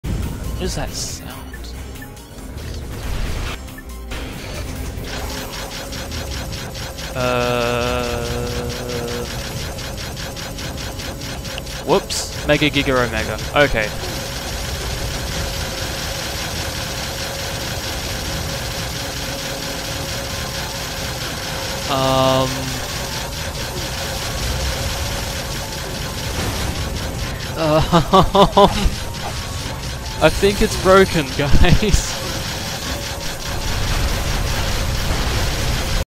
Megaturrets have a rave
Not sure if this is an emulation bug or if it was in the original game.